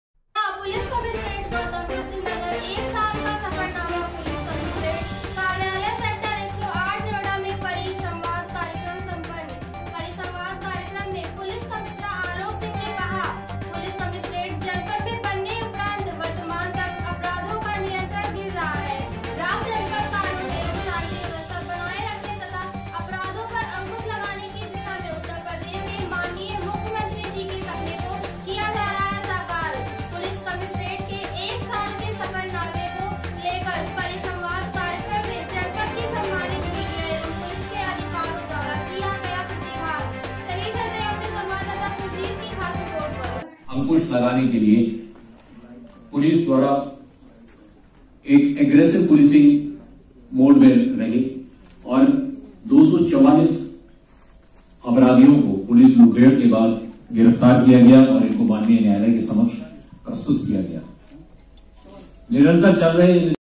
पुलिस कमिश्नरेट गौतम बुद्ध नगर का 1 साल का सफरनामा पुलिस कमिश्नरेट कार्यालय सेक्टर 108 नोएडा में
नोएडा

पुलिस कमिश्नरेट गौतम बुद्ध नगर का 1 साल का सफरनामा पुलिस कमिश्नरेट कार्यालय सेक्टर 108 नोएडा में परिसंवाद कार्यक्रम संपन्न-

 परिसंवाद कार्यक्रम में पुलिस कमिश्नर आलोक सिंह ने कहा पुलिस कमिश्नरेट जनपद में बनने के उपरांत वर्तमान तक अपराधों का निरंतर गिर रहा है ग्राफ जनपद में कानून एवं शांति व्यवस्था बनाए रखने तथा अपराधों पर अंकुश लगाने की दिशा में उत्तर प्रदेश के माननीय मुख्यमंत्री जी के सपने को किया जा रहा है साकार, पुलिस कमिश्नरेट के 1 साल के सफर नामे को लेकर परिसंवाद कार्यक्रम में जनपद की सम्मानित मीडिया एवं पुलिस के अधिकारियों द्वारा किया गया प्रतिभाग इंडिया समाचार से रिपोर्टर सुरजीत कि खास रिपोर्ट